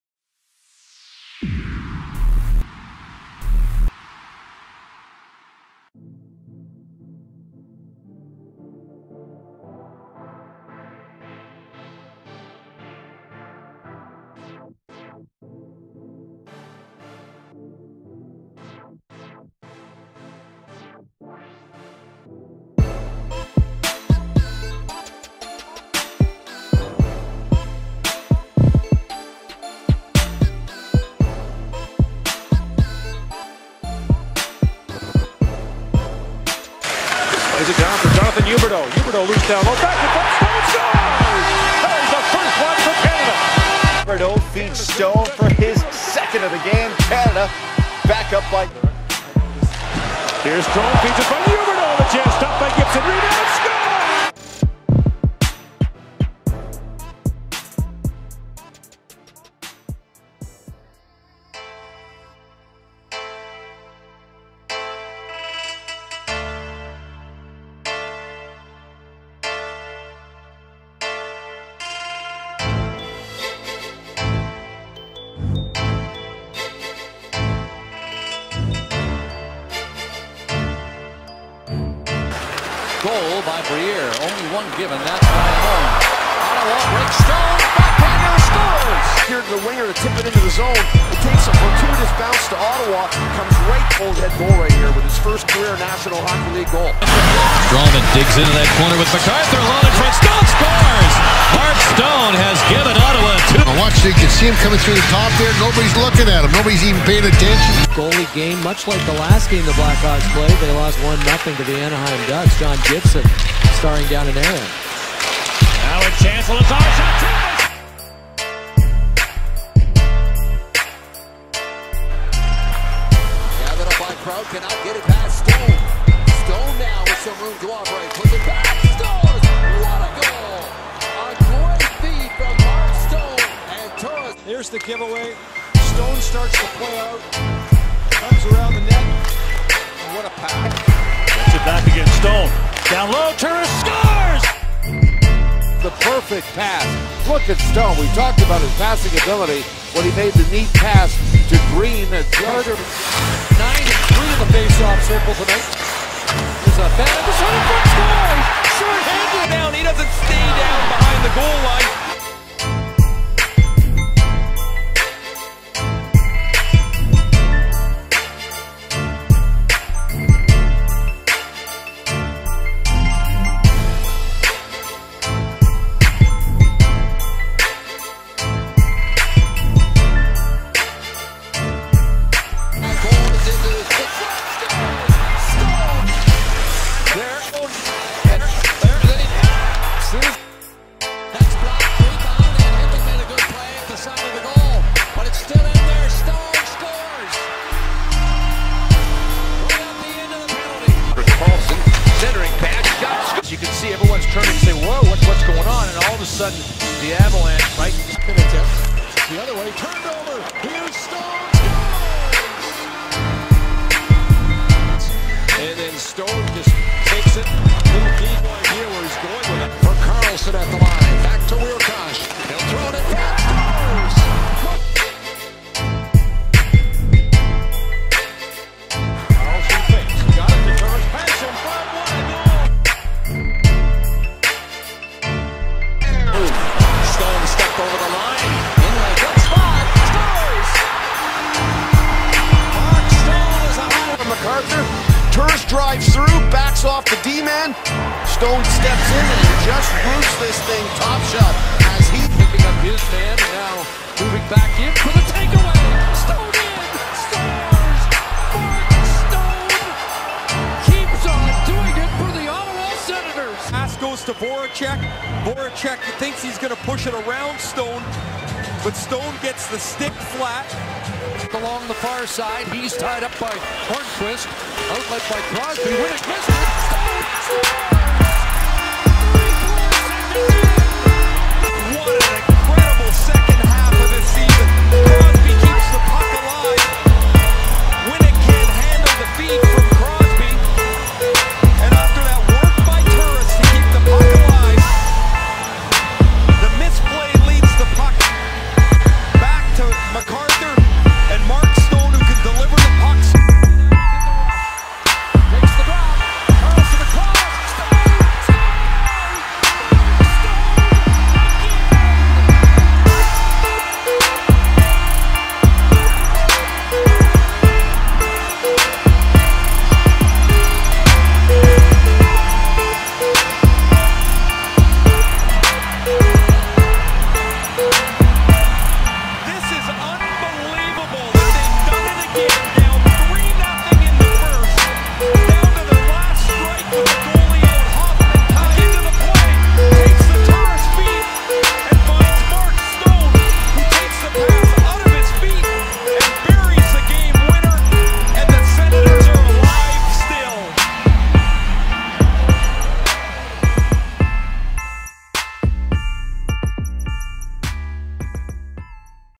#61 Mark Stone | "NHL's Best Stick"
Containing highlight footage of the most underrated 2 way player in the game today. The pickpocket artist leads the entire NHL in takeaways and possesses a very impressive hockey IQ. He was robbed of the Calder Trophy in 2015, but there are many reason to believe he will take home the Selke trophy in the years soon to come.